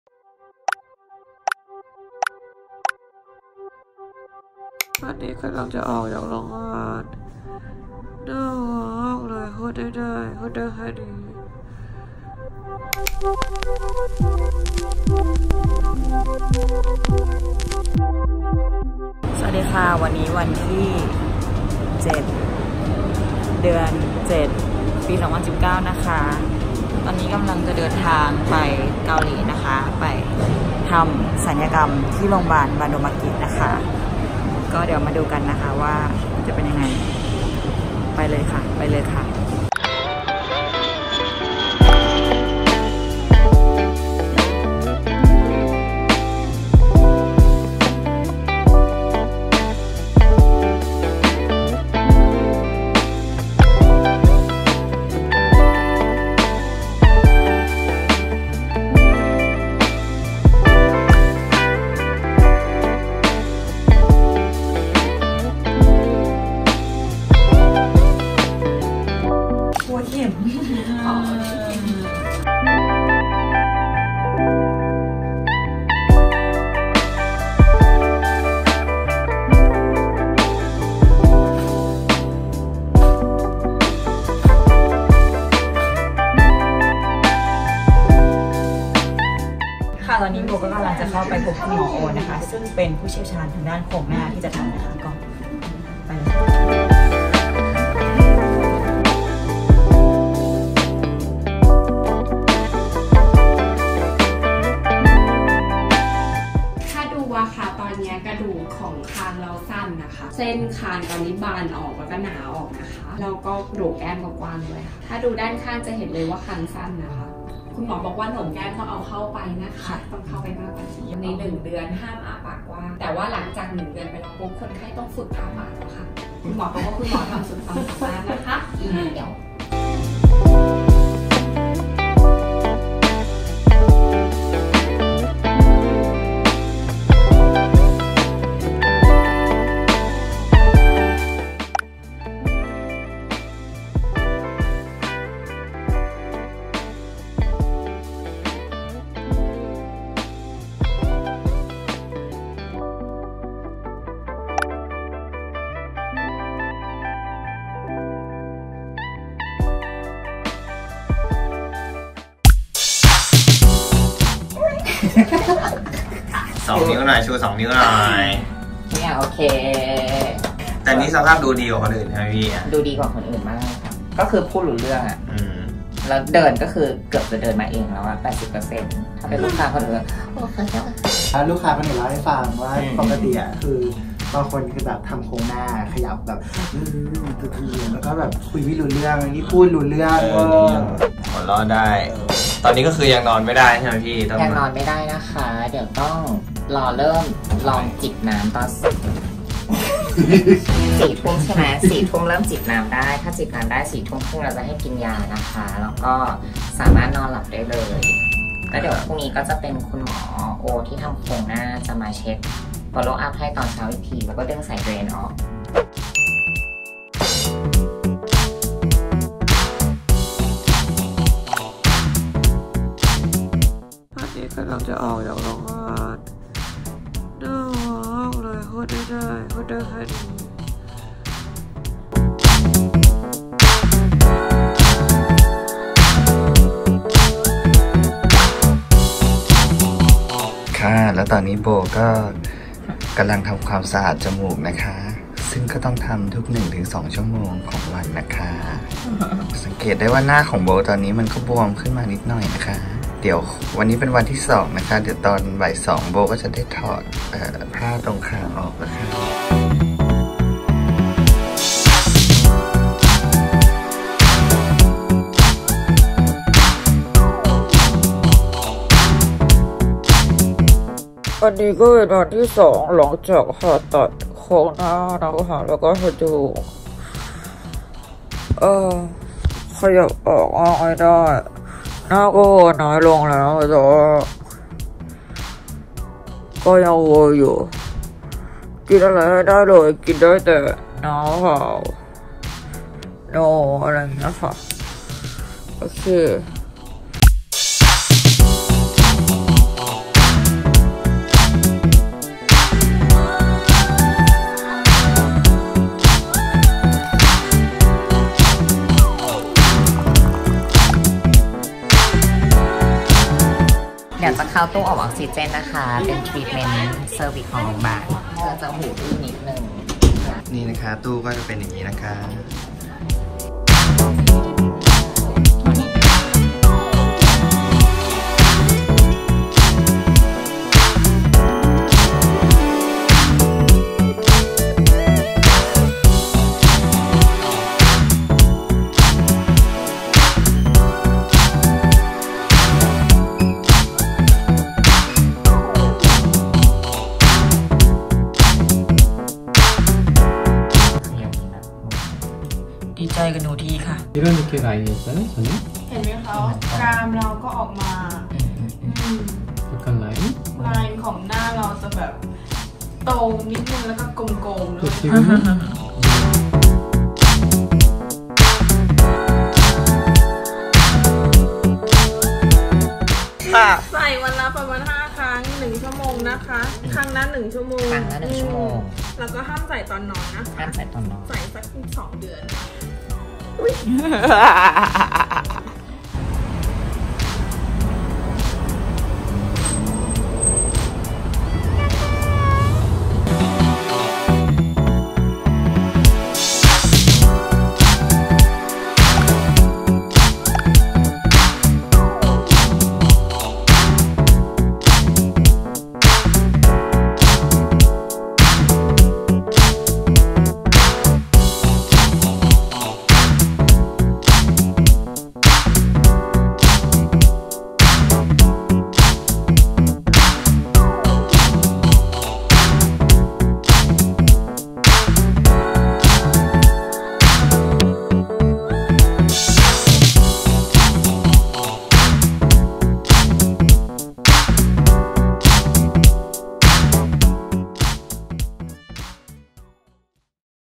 ศัลยกรรมบนใบหน้าครั้งแรกในชีวิต ที่บาโนบากิ ประเทศเกาหลี! | BOWKYLION
คนที่มีความสุขที่สุดในโลกนี้ คือคนที่พอใจในสิ่งที่ตัวเองเป็น : )

ปรึกษา​ศัลยกรรม​เกาหลี
PJ beauty Journey เอเจนซี่​ศัลยกรรม​เกาหลี

โรงพยาบาล​ศัลยกรรม​บาโนบากิ
Line :  @thaibanobagi (มี@ด้วยค่ะ)